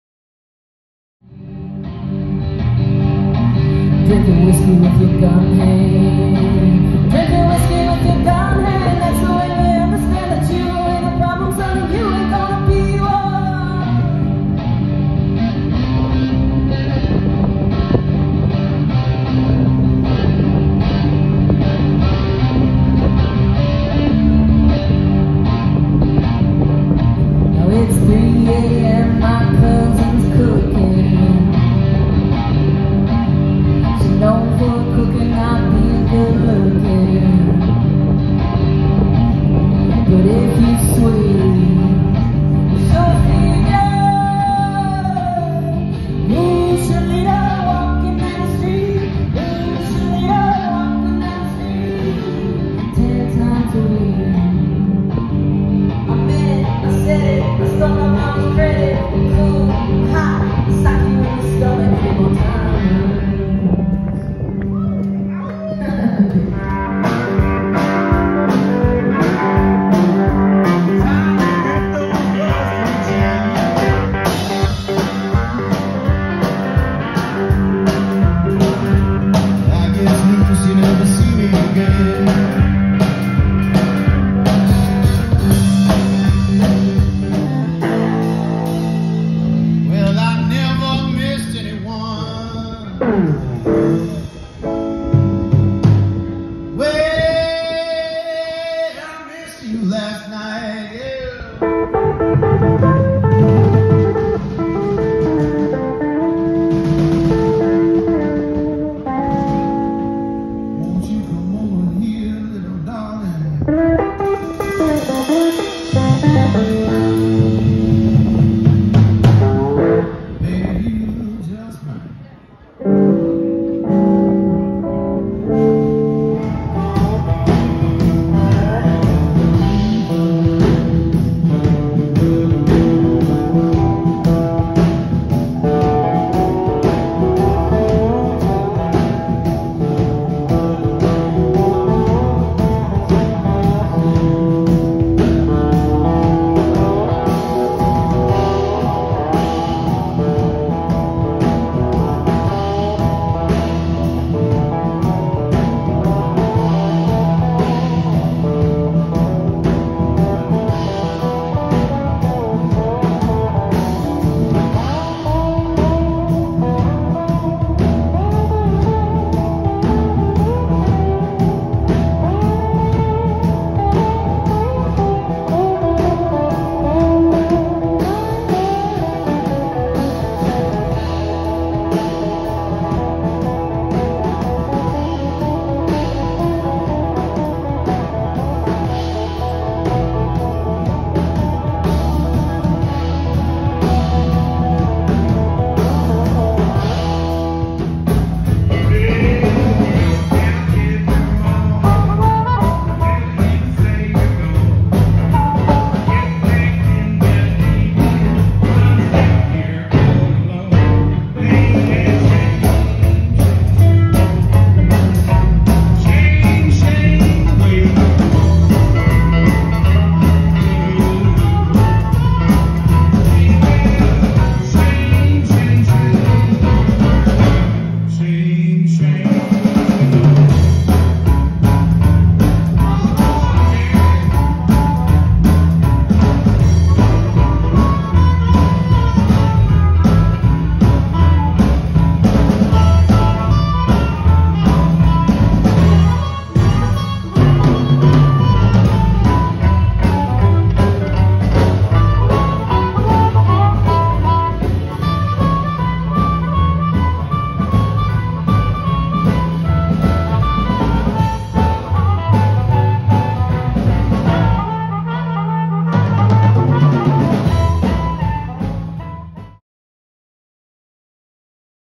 The 2024 Republic of Texas CBG Festival Recap!
This past Saturday, the 2024 Republic of Texas Cigar Box Guitar Festival was held in Blanco, TX. It was hosted at the Andalusia Whiskey Company & Speak Easy Grill. I attended and had a great time. Here's a quick video recap of the festivities including 4 musical acts and a harmonica workshop.